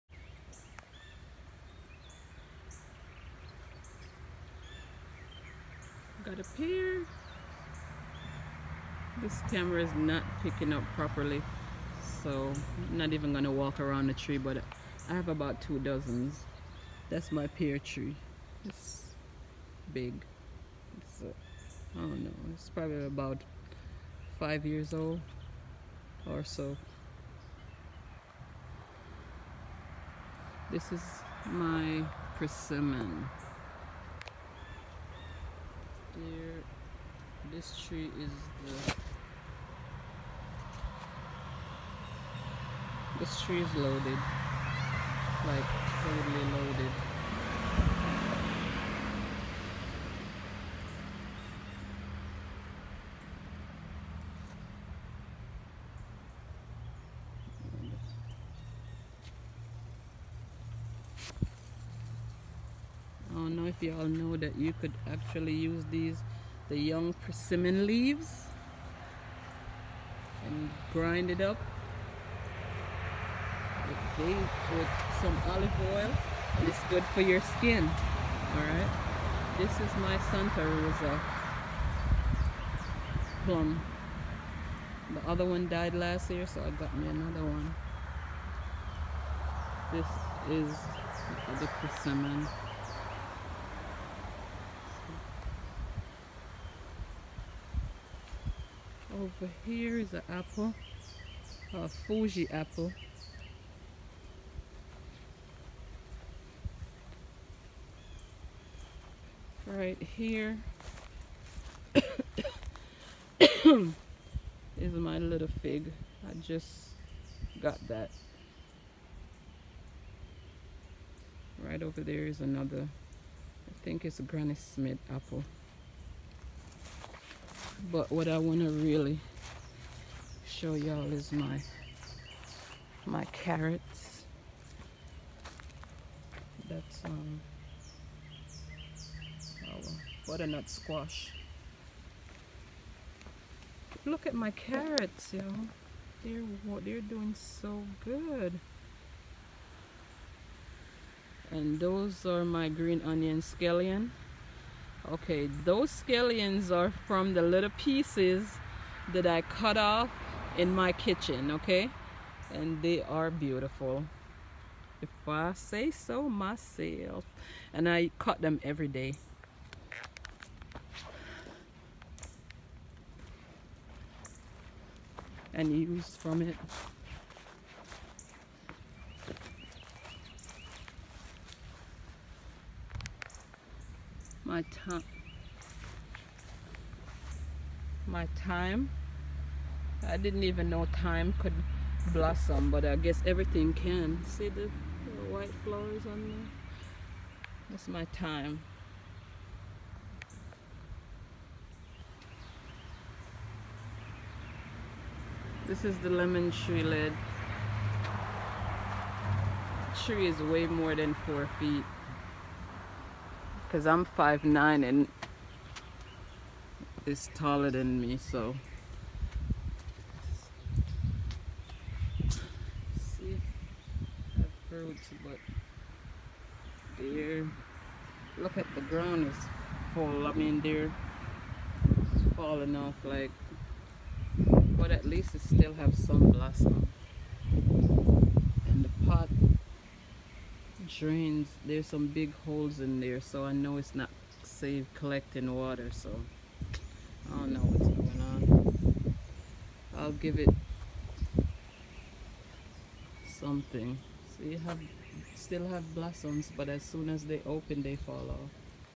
my fruit trees
A small tour of my garden and fruit tree 🌳